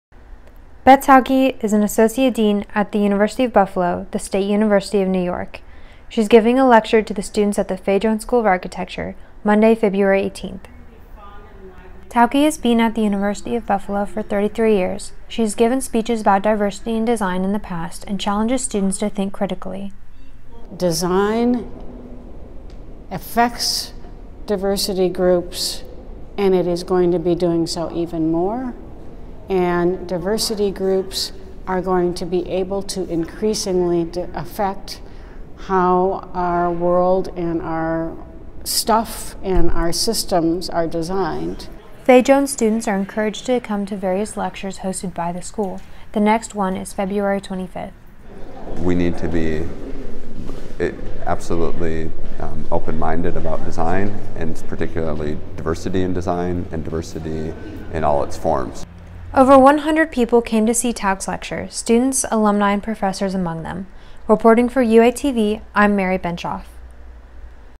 Beth Tauke's Environmental Design Lecture at the University of Arkansas
February 18, Beth Tauke, associate dean and associate professor in architecture at the University of Buffalo - the State University of New York - came and spoke to Fay Jones architecture students at the U of A.